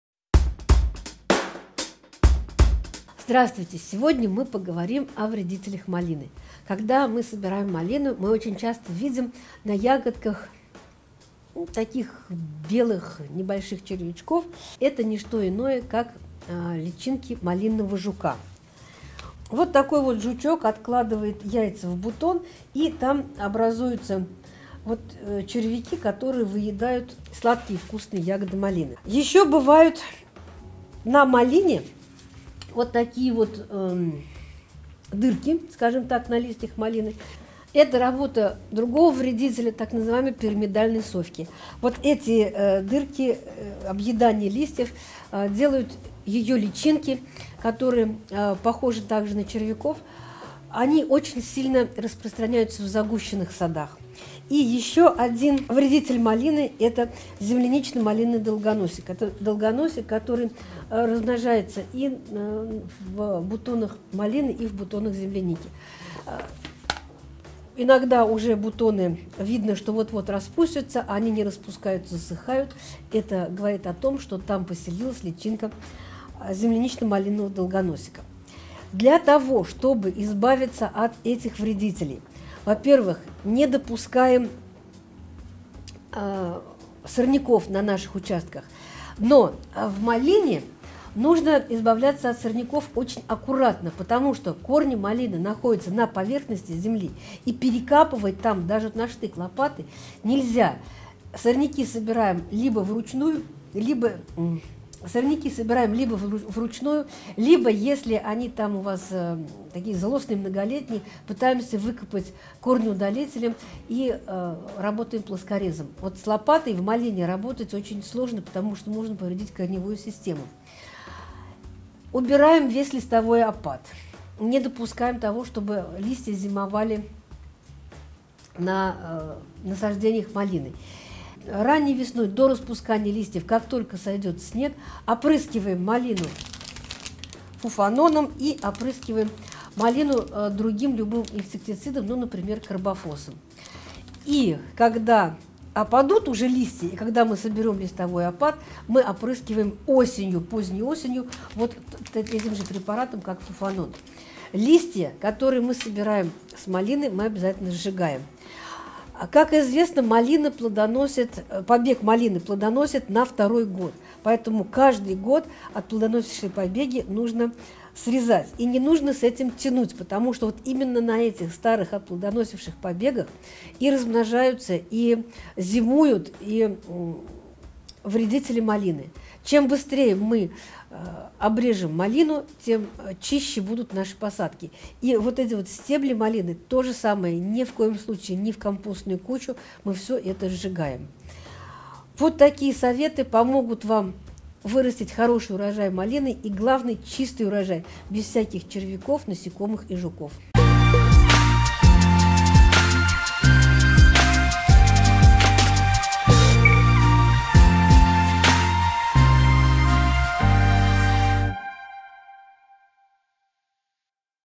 Вредители малины и борьба с ними 🌿 Фуфанон ✅ Карбофос
Расскажем какие вредители обитают в малине и как с ними бороться с помощью препаратов:
✅ Фуфанон
✅ Карбофос

• Выбрать и купить препараты для борьбы с вредителями малины, прочесть отзывы покупателей, инструкцию пользователя и подробные характеристики можно на сайте